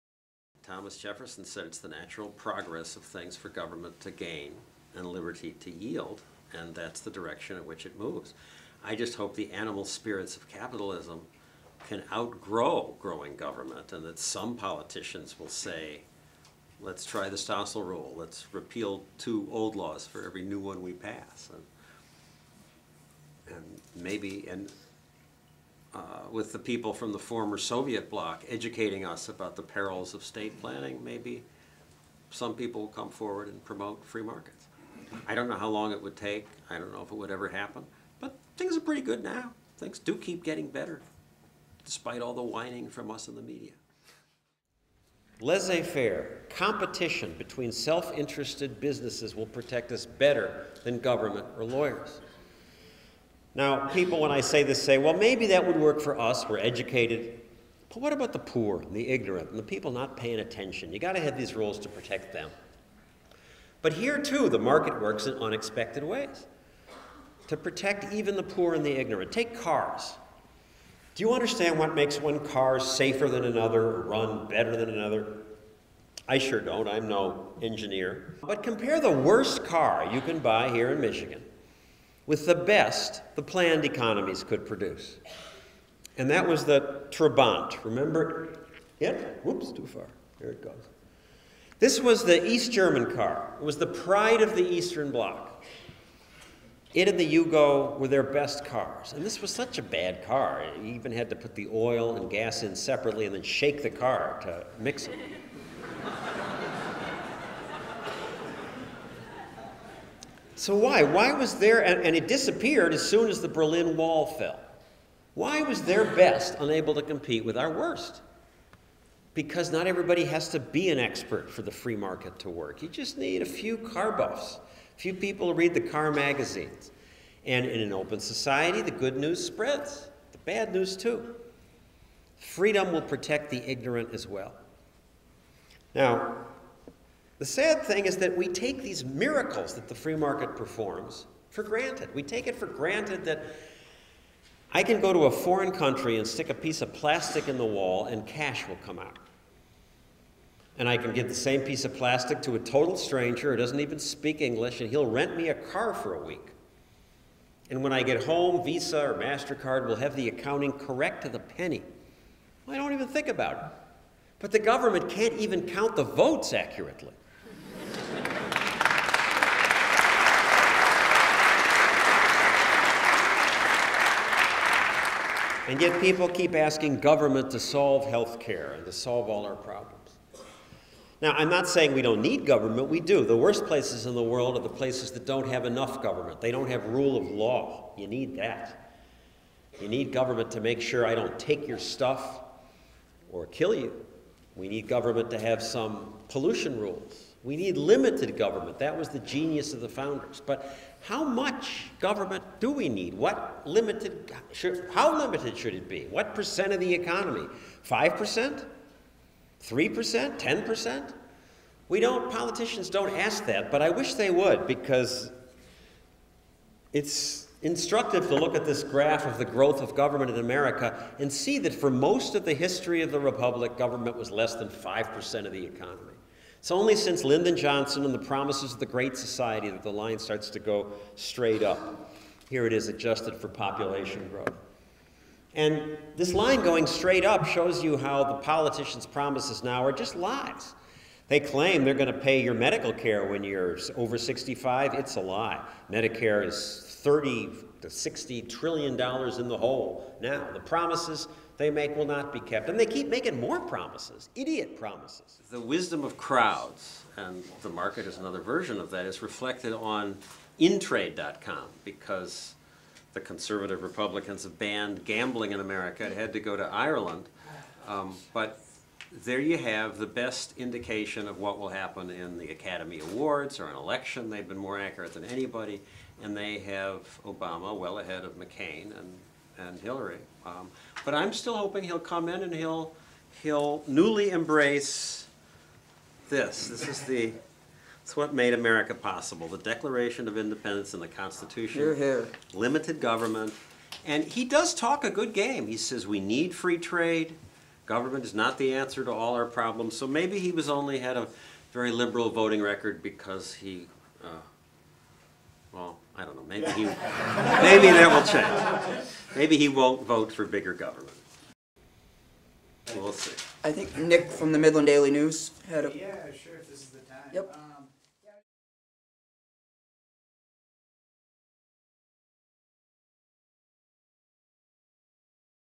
John Stossel in Michigan
John Stossel was at Central Michigan University on March 19th. This is a clip of what he had to say.